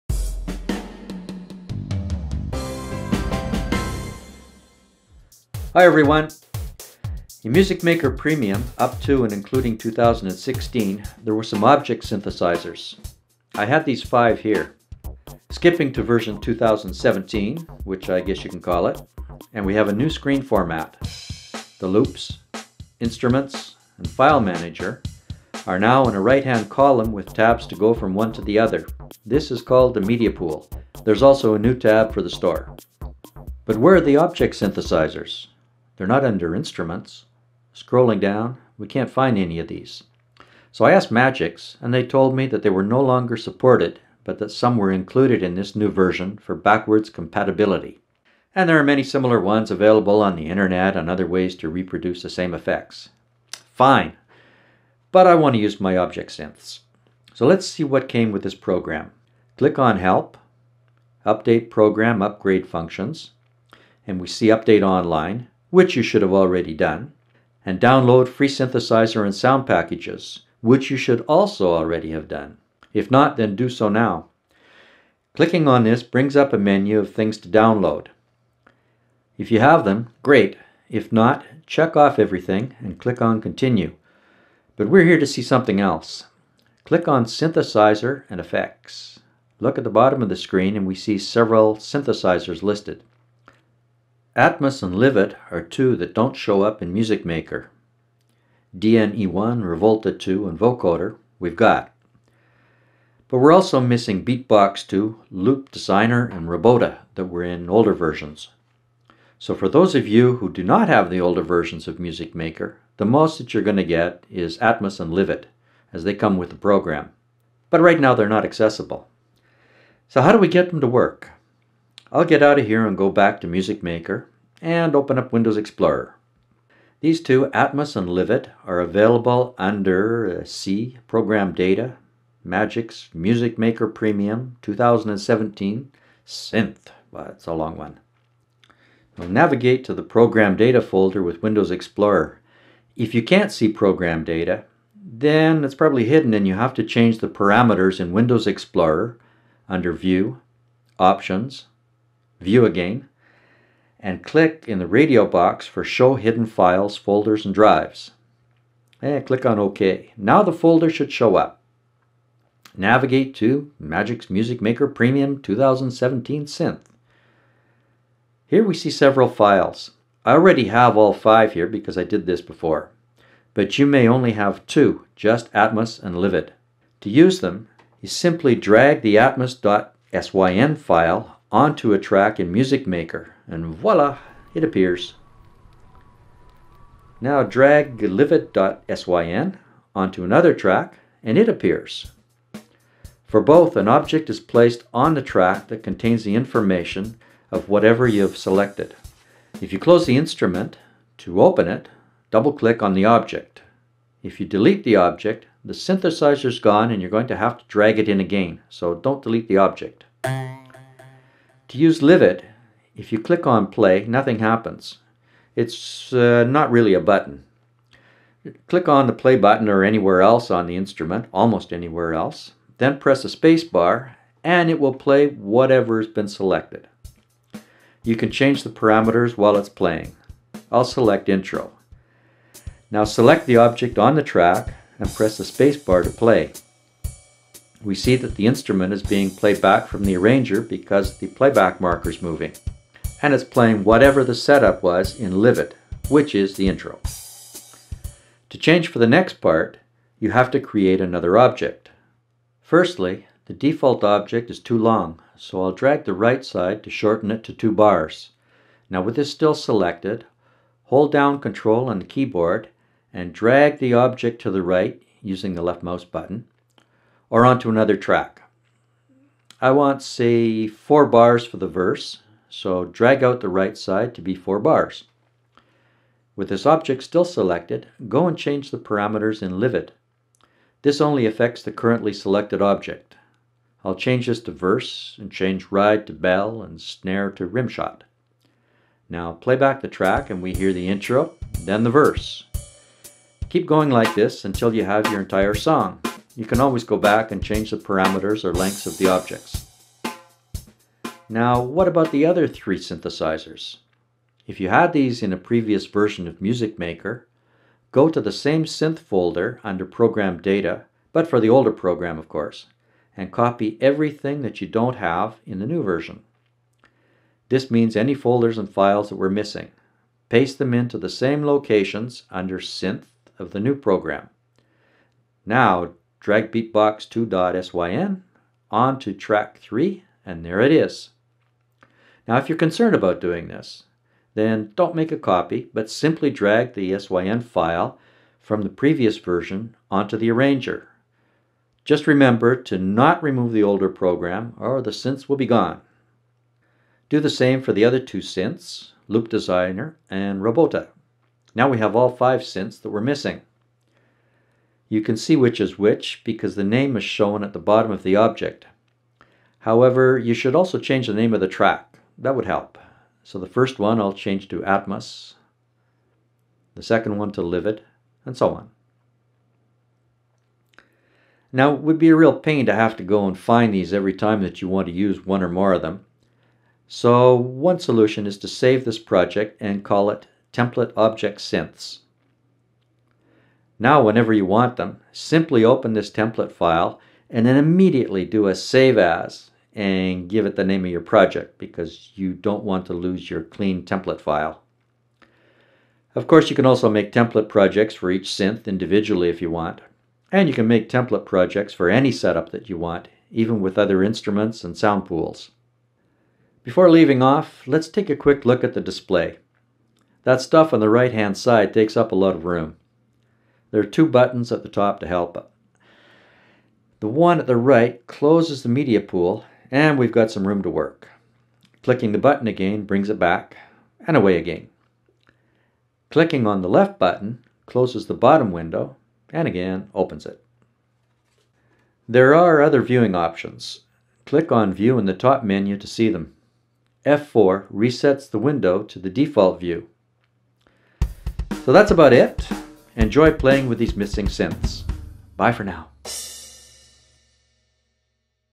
Tutorial 016 MusicMaker 2017 Find and Use the Hidden Object Synthesizers
In MusicMaker Premium (2017), the Object Synthesizers, Atmos, Livid, BeatBox2, LoopDesigner and Robota are no longer accessible. This tutorial shows how to get them back if you have an older version of MusicMaker, and shows how to get at least Atmos and LiVid if you don't have an older version.

NOTE: This trick does not work in later versions of Music Maker, like 2021 and 2022, which are 64 bit versions.